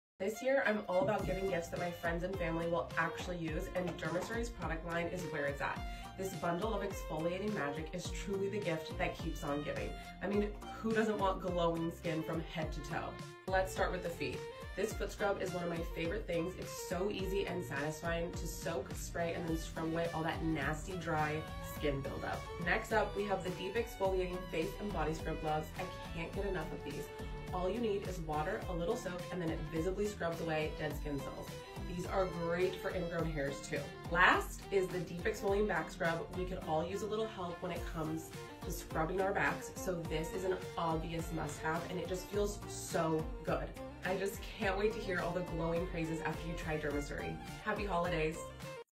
Must Have Stocking Stuffers
Stocking Stuffer gift ideas for your friends and family. ✨⁣